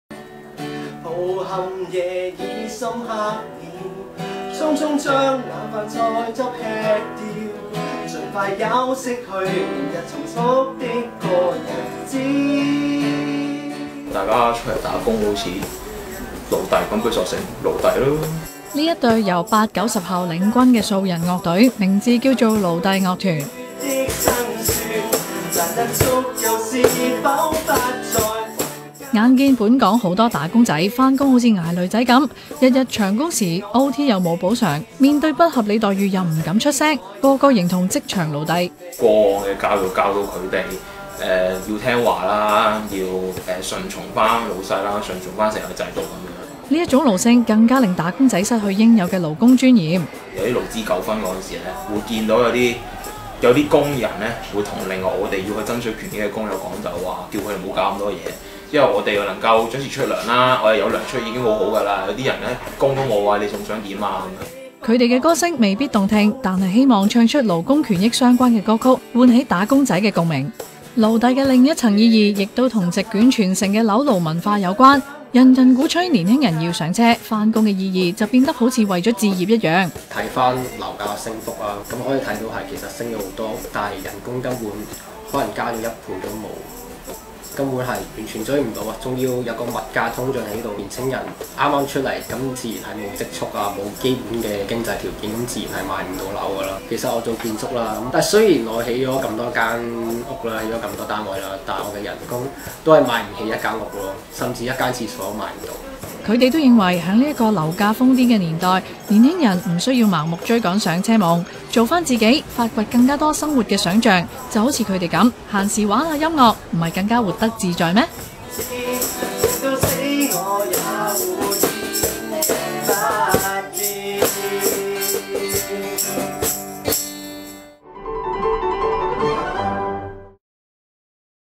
90後夾Band組勞弟樂團 唱盡打工仔買唔起樓哀歌
【文化籽：九十後話】
「準時出糧已經係恩賜。」這句話不是甚麼劇集的台詞對白，卻是活生生出自本港打工仔之口，聽入耳，絕對當頭棒喝，香港人上班如奴隸是常識吧，但變得如此卑微，卻令人心酸又無奈。這五個八九十後年輕小伙子，日前膽粗粗埋班組成「勞弟樂團」，素人夾band的初衷，不為掌聲，卻志在唱盡香港職場辛酸及社會畸態，將港人的奴隸本色展現人前。他們滿腔熱誠，賣力地唱，只求喚醒昏睡的一群。